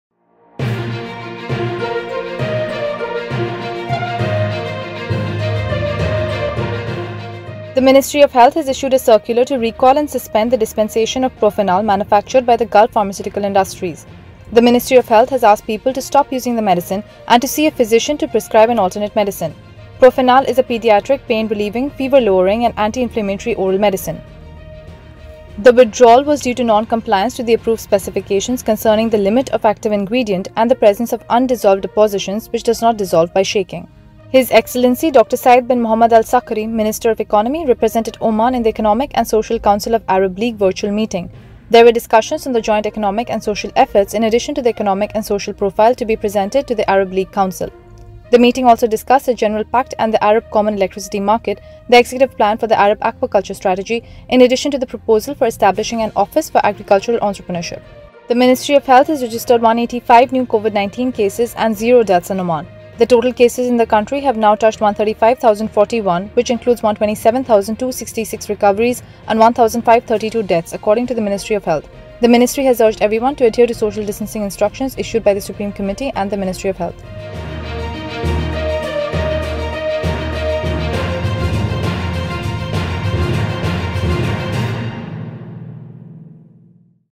Maghrib News Wrap Up
1. Ministry of Health recalls Profinal
2. Oman takes part in the Arab League Meet
3. New cases of COVID-19